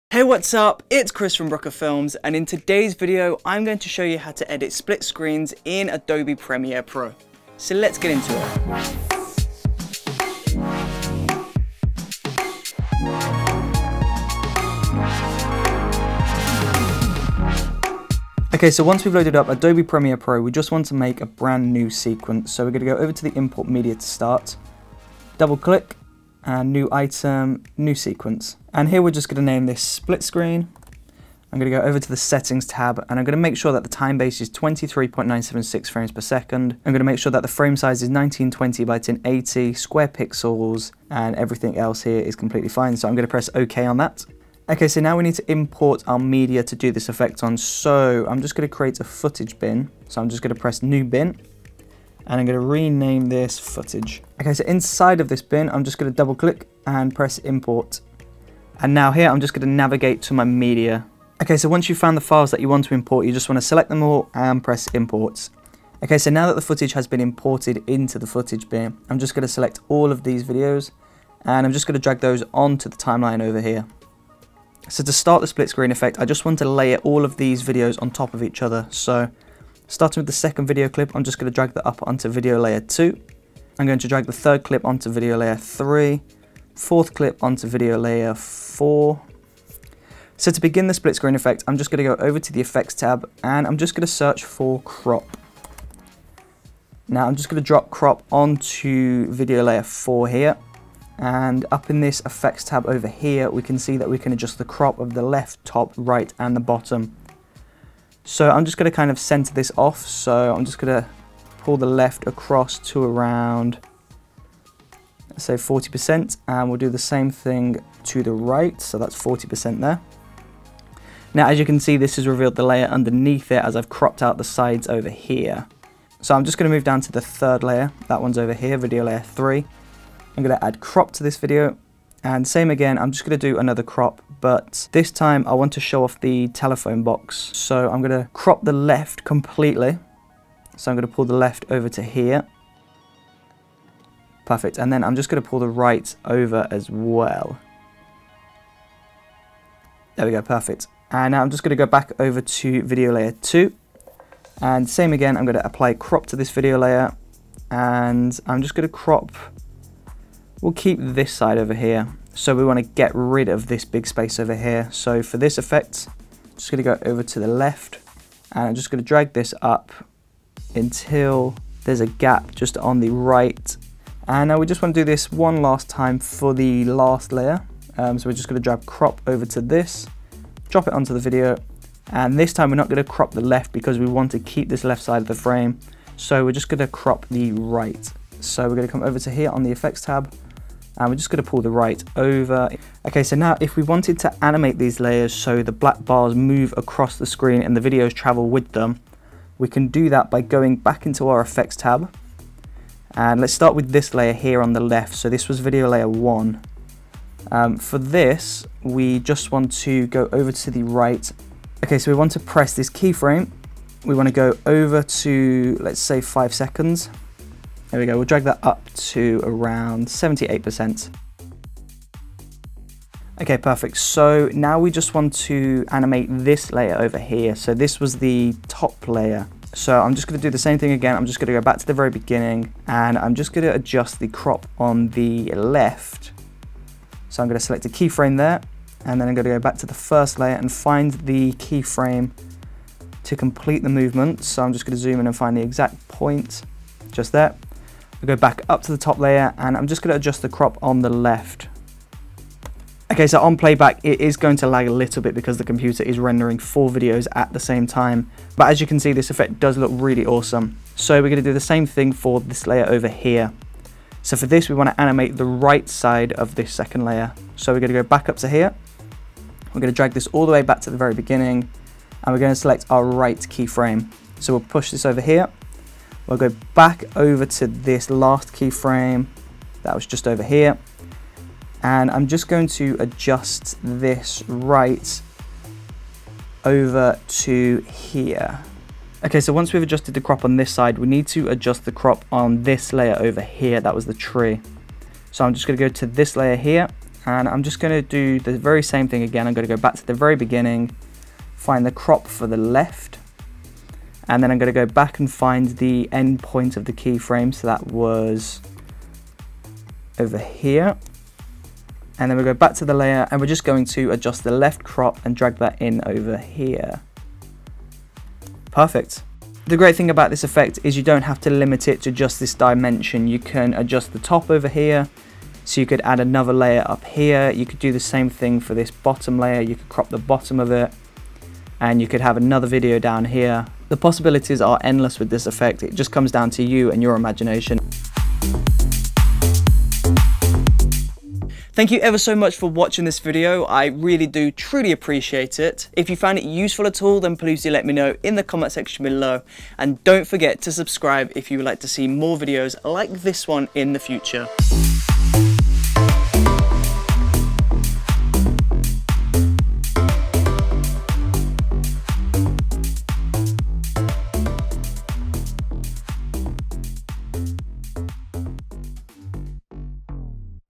Adobe Premiere Pro Tutorial: How to Create Split Screen (Side by Side) Video Effect
Learn how to create split screens in Adobe Premiere Pro.